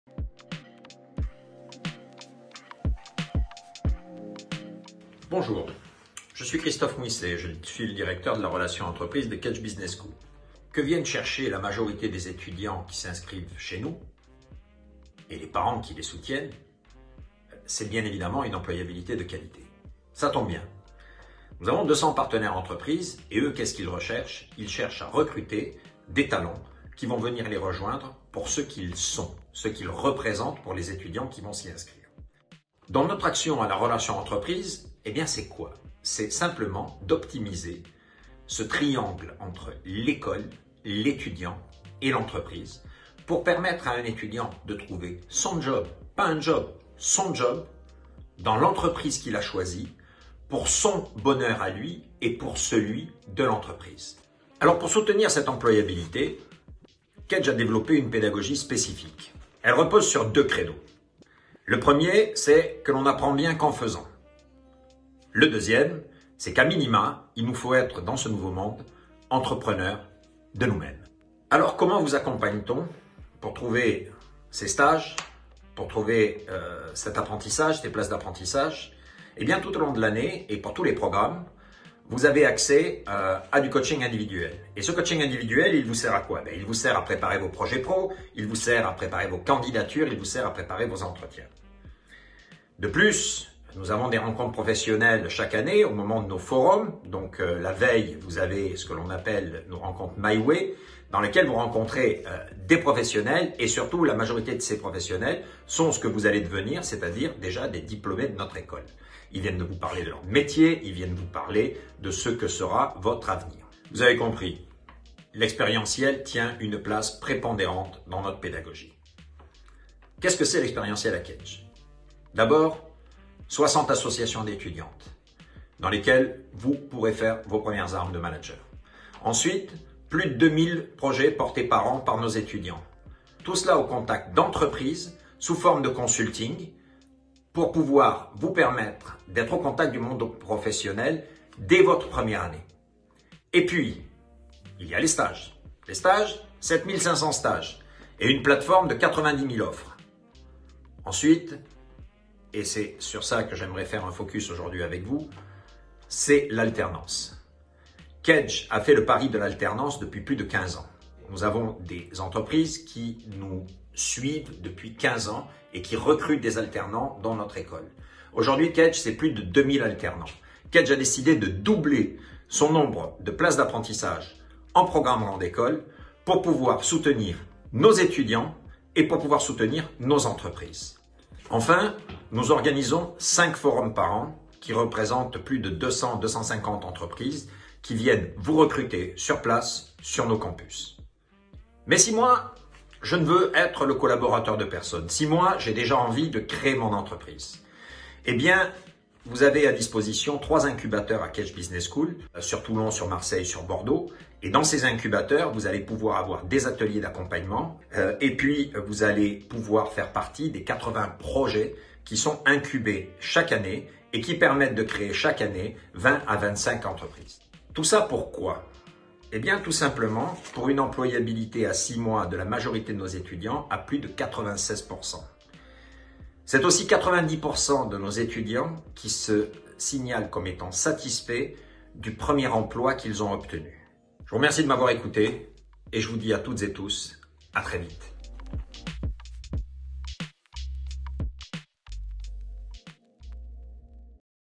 Christophe Mouysset - L'employabilité
Hebdo by Kedge : Message de Christophe Mouysset, Directeur des Relations Entreprises, à notre communauté. Il nous présente l'employabilité et l'expérience professionnelle permises par l'école à nos étudiants.